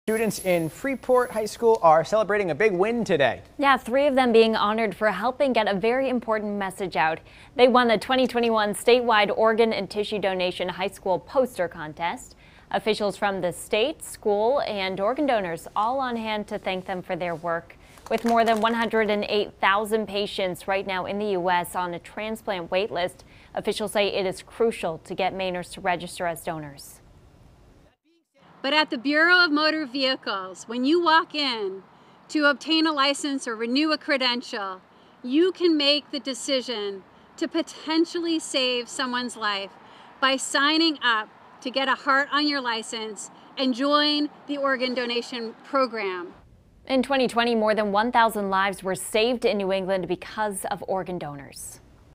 Freeport High School students win organ, tissue donation poster contest
Three Freeport High School students were named the winners of the 2021 State Organ and Tissue Donation Poster Contest